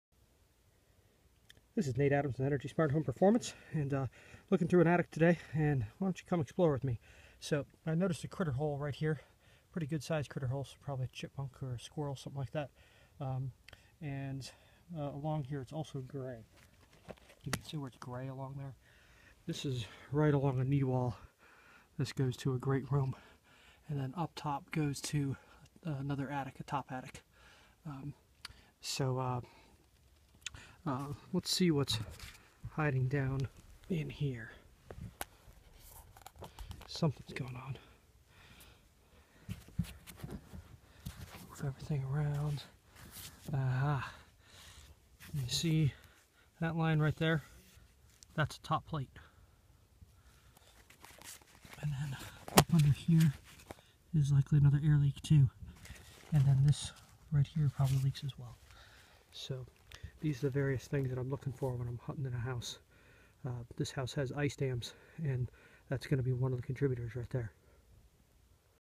Discovering leaks under a knee wall that are contributing to ice dams and icicles.
Come explore with us! Watch as we literally dig to find problems to solve in a home with serious icicles and ice dams. We created a plan just to solve these issues for him, not a comprehensive plan.